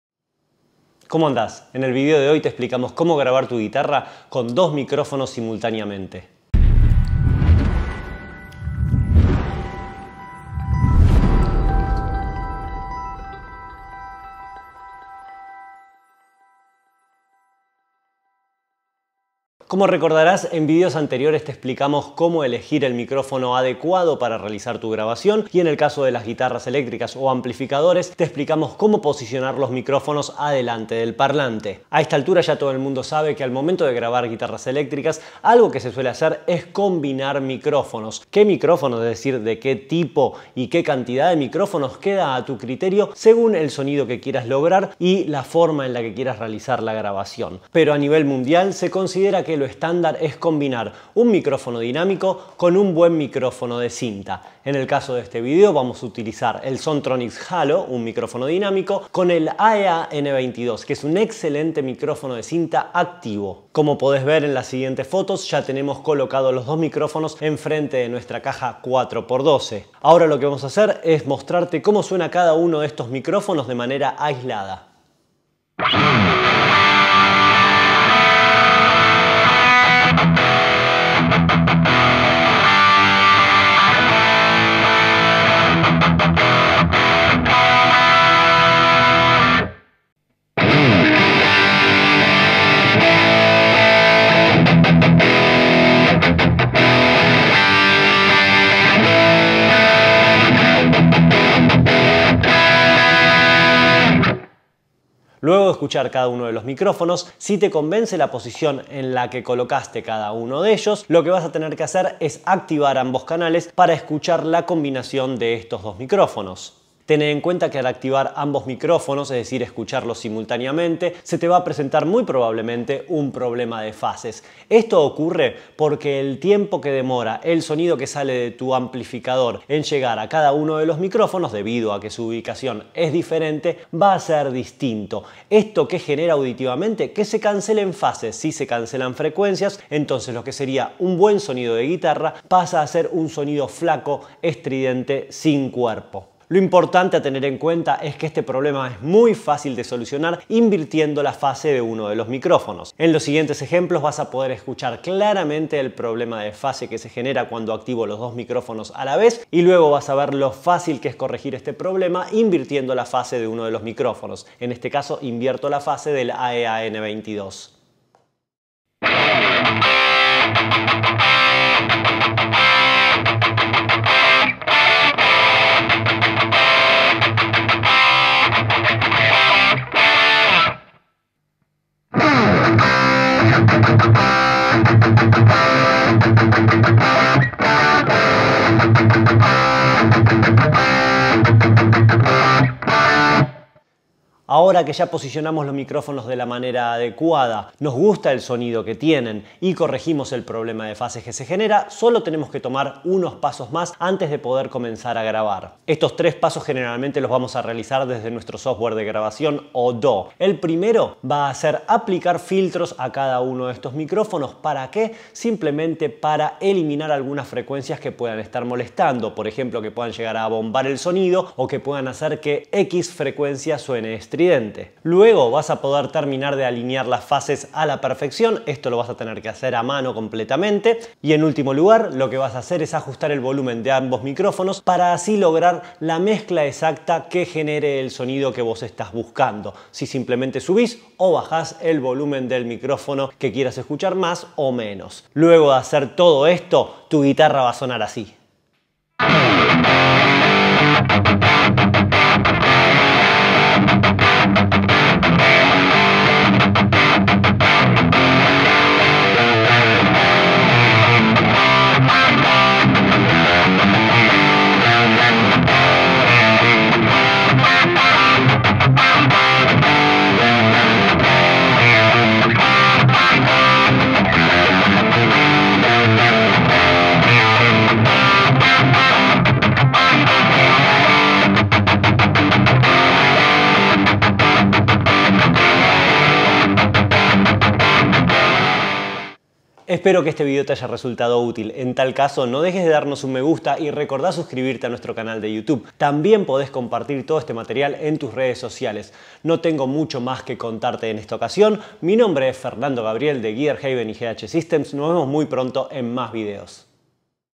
¿Cómo grabar guitarras con 2 micrófonos?
En este video, te explicamos todo lo que tenés que saber al momento de grabar amplificadores con 2 micrófonos simultáneamente. No te lo podés perder!!!